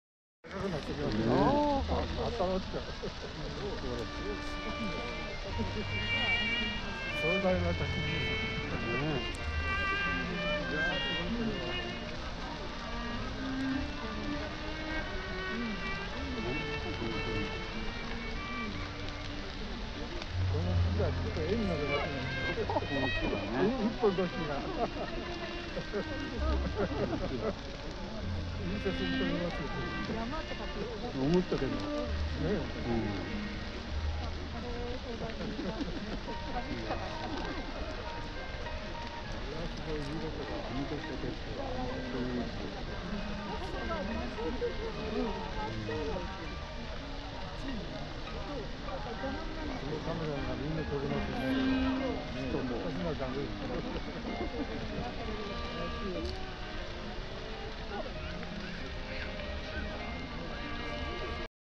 若草山の山焼き(２０２４)
若草山で毎年１月末に行われる山焼きの映像です。
今年は、雨が降る事も無く、風も無かったので、きれいな映像が取れました。